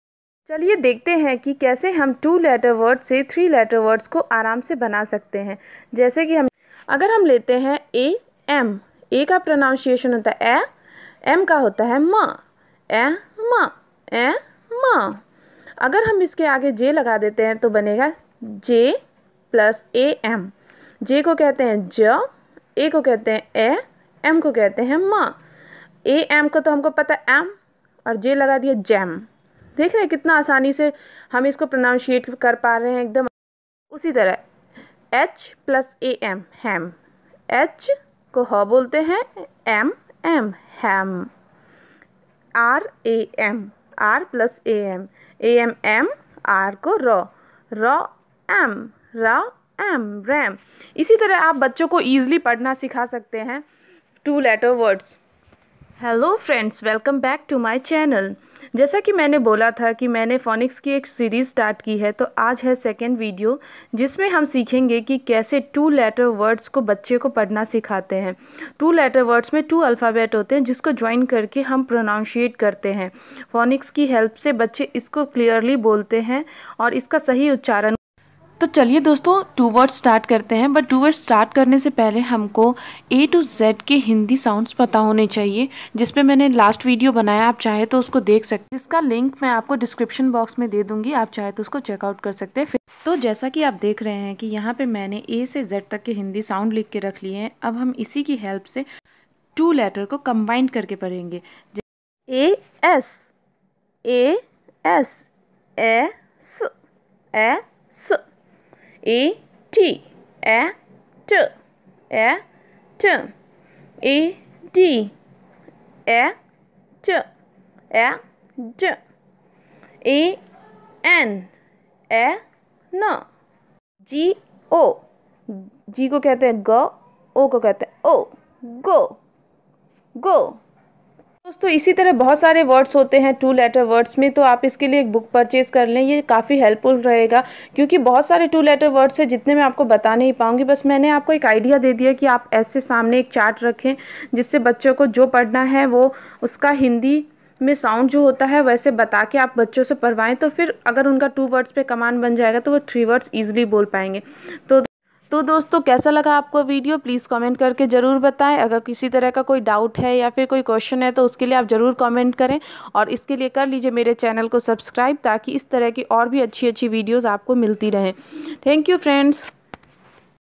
बच्चों को कैसे two letter words पढ़ना सिखाये | two letter words phonics
Hii friends

Aap is video me dakhoge ki kaise do alphabet ko join kr ke phonics ki help se hm two letter words bnate h oR pronounces krte h

👆is video ki help se chart prepare kr ke wall pE lgaye